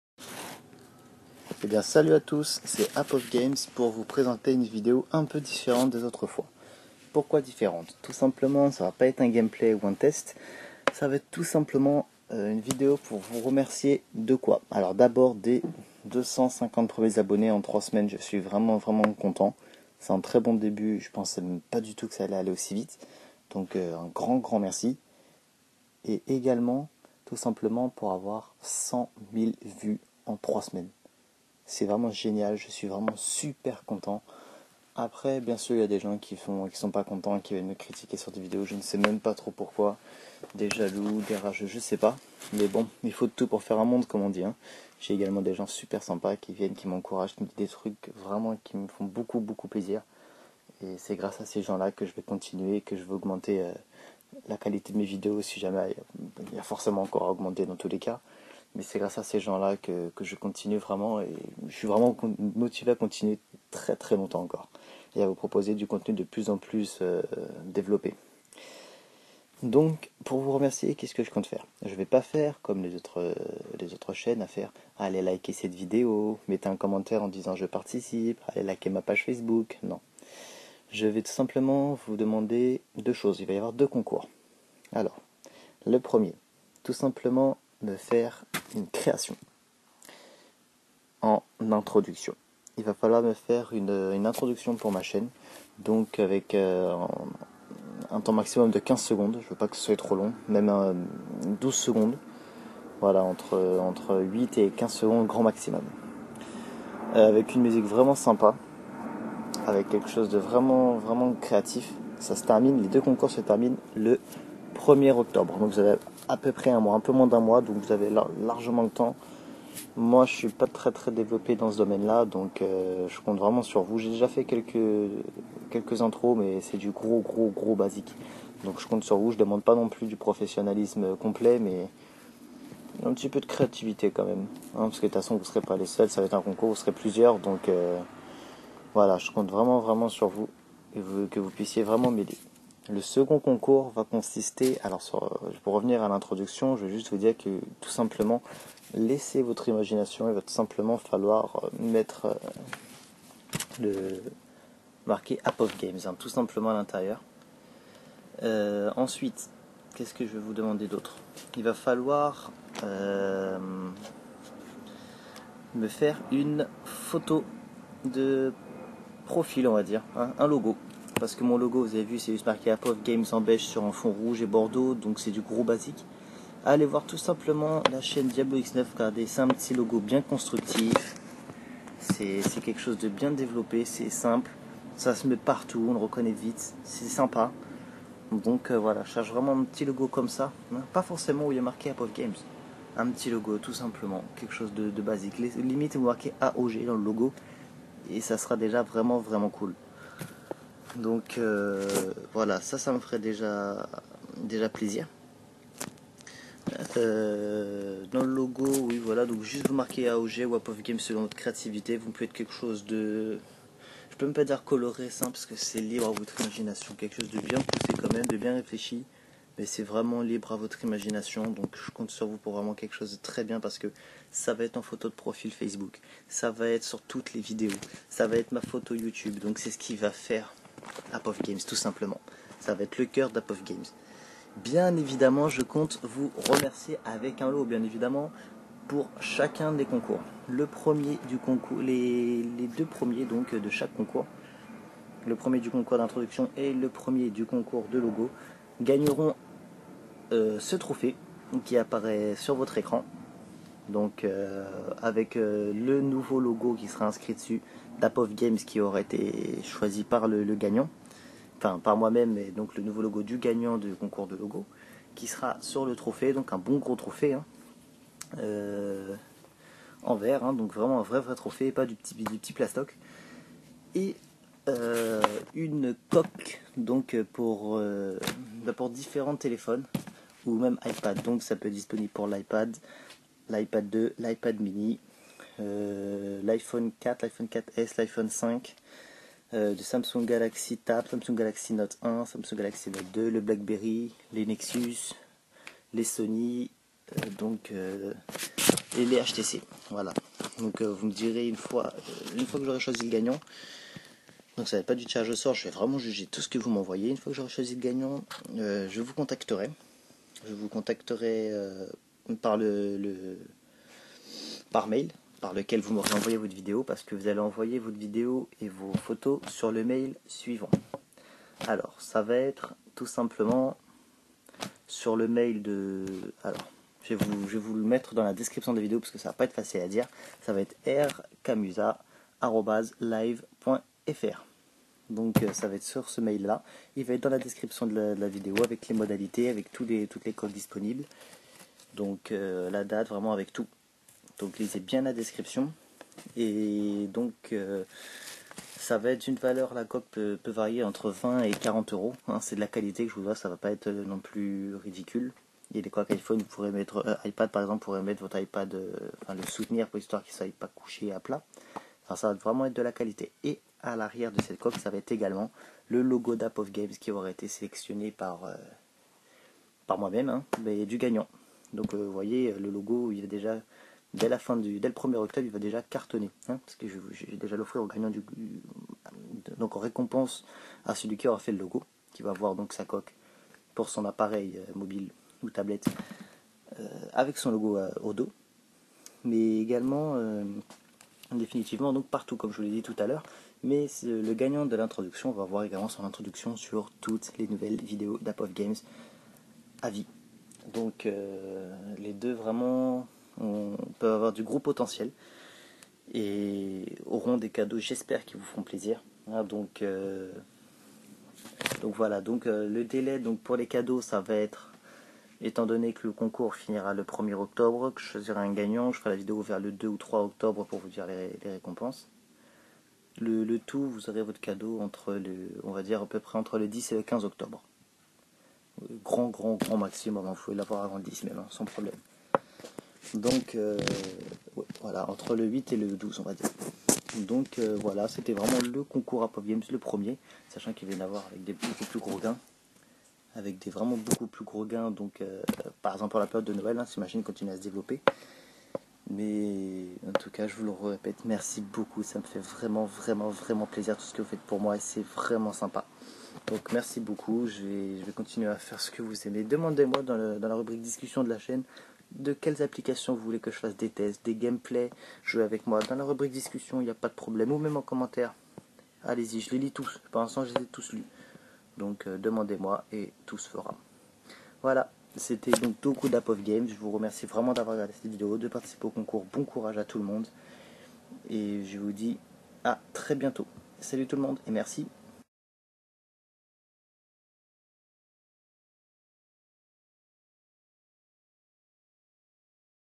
AOG - Grand Concours - Trophée et coques à gagner !
Règlement du concours :

1- Création d'une introduction : Durée maximale de 13 secondes. Il faut que l'on sache des l'introduction que l'on se situe sur la chaine AOG. Une musique pas trop forte ou imposante, mais qui plait tout de même/

2- Création d'un logo : Il sera la photo de profil Facebook, la photo de profil YouTube, et inscrit sur toutes mes vidéos. Il sera également inscrit au laser sur les trophées à gagner, et sur les coques. Il faut quelque chose de simple mais efficace. Prenez exemple sur DiabloX9, pas un gros trucs, mais quelque chose de sympas. Ne confondez pas logo et image (comme mon logo actuel par exemple).

A gagner :

- 2 Trophées (voir image dans la vidéo) avec inscrit au laser le nouveau logo tout frais d'AOG (ne vous inquiétez pas il n'y aura pas marquer Menuiserie etc)
- Des coques de protections simple en couleurs mais chargé en solidité et très confortables, avec au dos le nouveau logo tout frais d'AOG (disponible pour Ipad, Ipad2, Ipad Mini, IPhone 4-4S-5, IPod, Nexus, Sony, HTC & BlackBerry)

Fin du concours, le 1er Octobre 2013

Si vous êtes l''heureux gagnant, je vous contacterai pour connaitre vos coordonnées de façon à recevoir votre cadeaux.